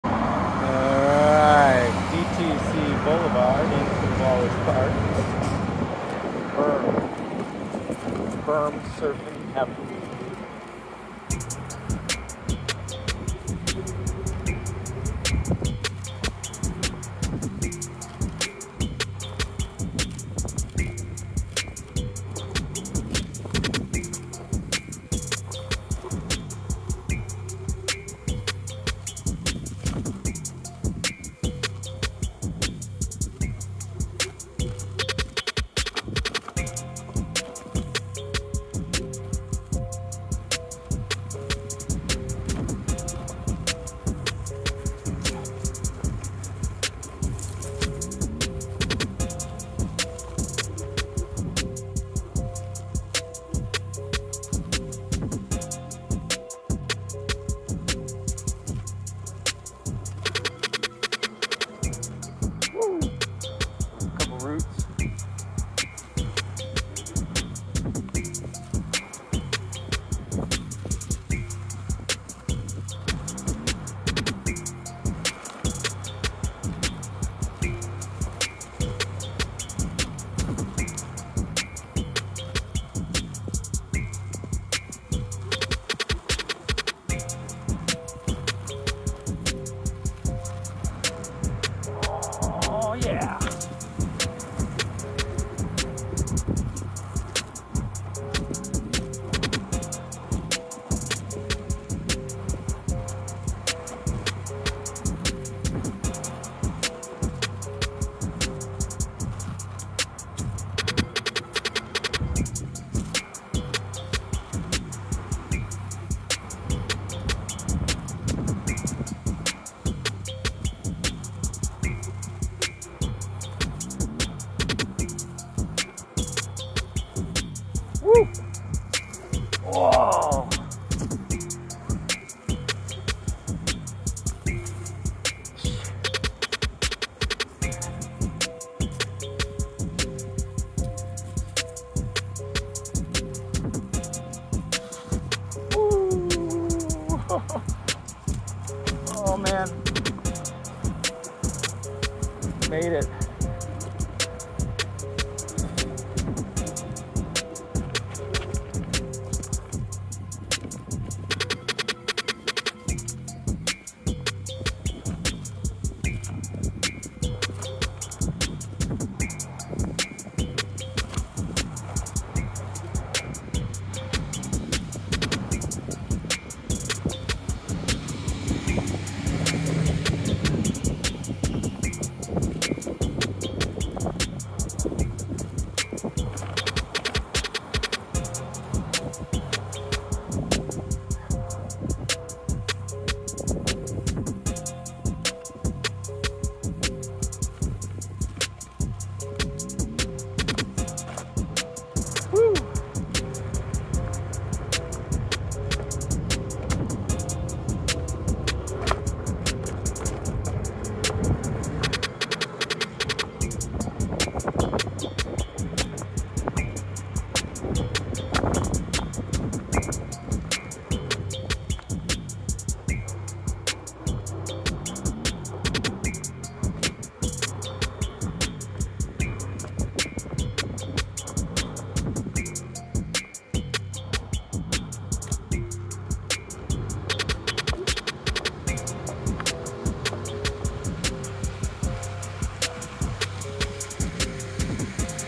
Onewheel Grass Surfing Long Berm at Wallace Park, Denver Tech Center, Colorado
This long grass berm alongside Goldsmith Gulch in Denver Tech Center is one of my favorites for Onewheel surfing. Handled Pixel5.